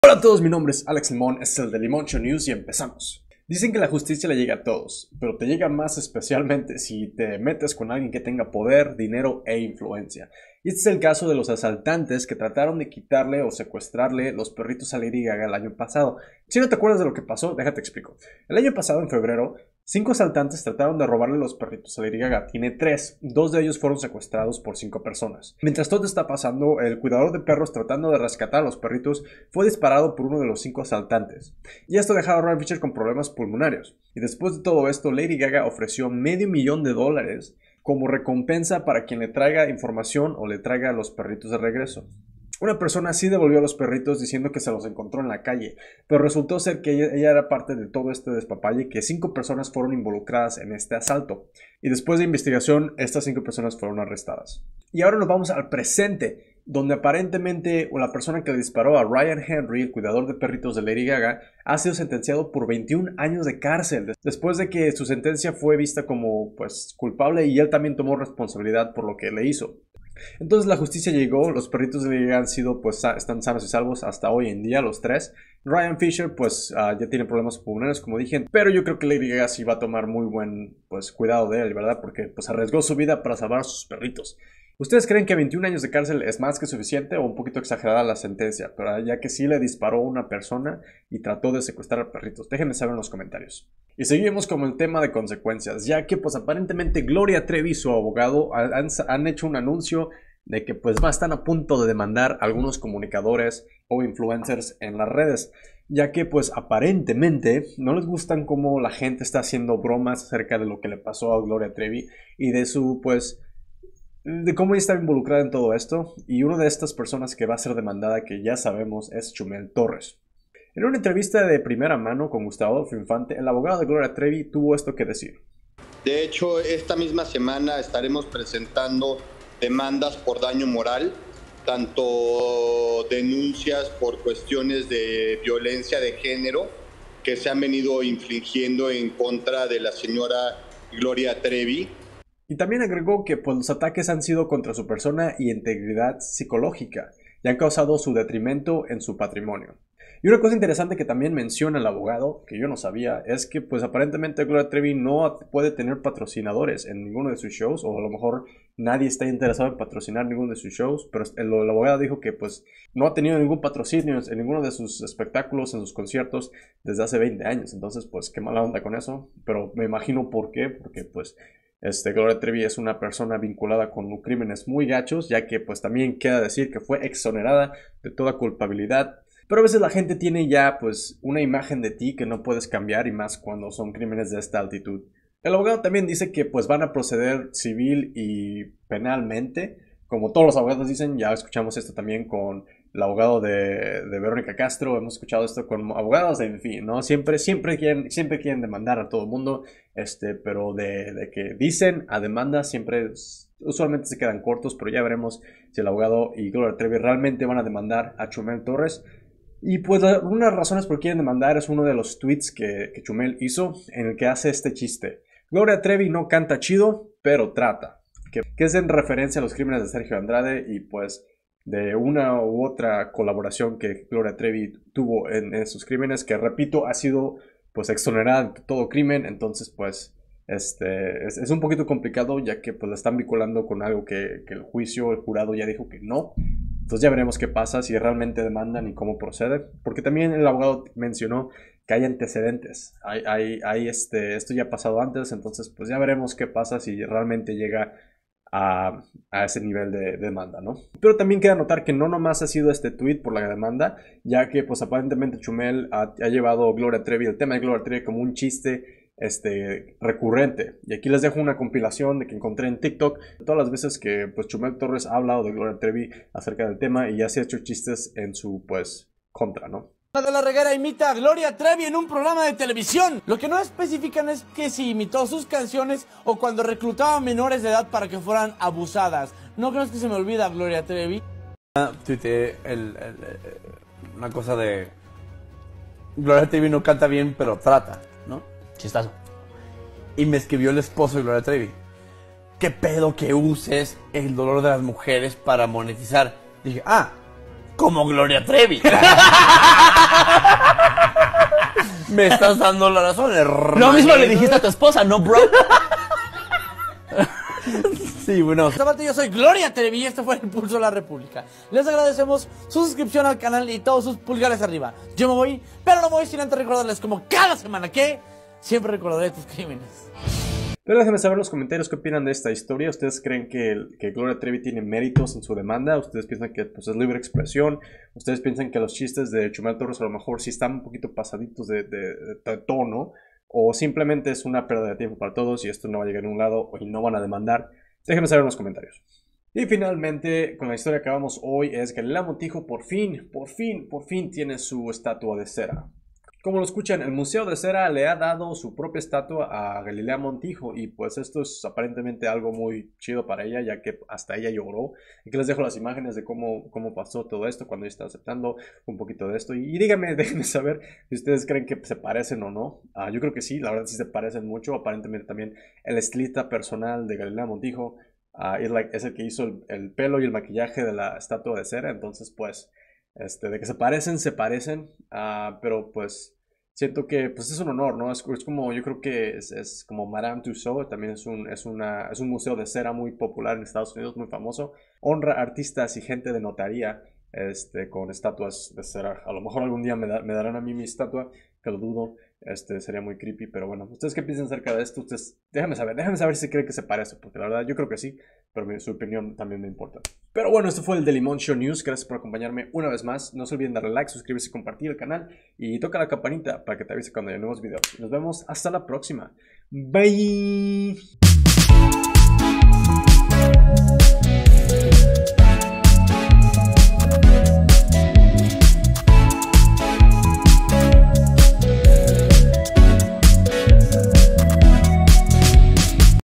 Nueva Demanda!! Gloria Trevi Demanda a Chumel, Justica Para Lady Gaga, Y Galilea Montijo Es De Cera?
Contenido en Ingles!!

Mis Cuentas! Sigueme!

Checa some merch!
Conoce a mi mama!